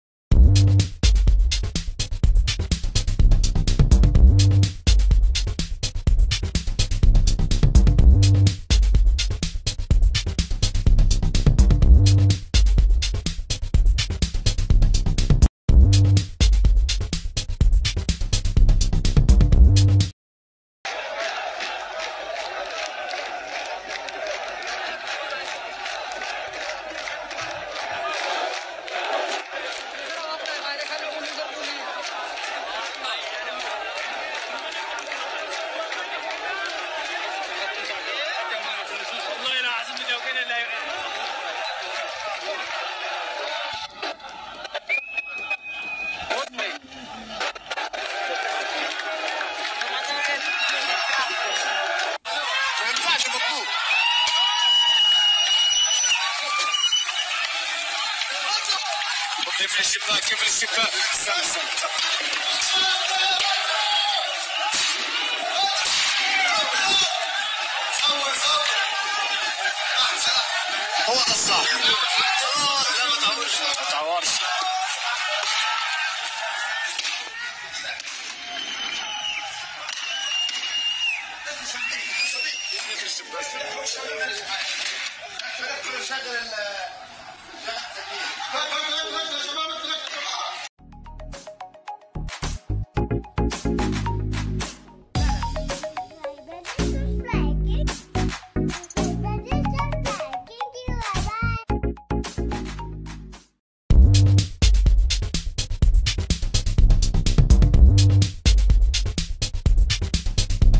Kuwait, Expat in Kuwait make chaos in quarantine center in Kabd, Police response to it.
It happened in Kuwait were expats made a chaos inside the quarantine area in Kabd, made the police response to it..in my part hope we will cooperate with the authorities because they were doing their very best to fight this pandemic. GOD BLESS us all.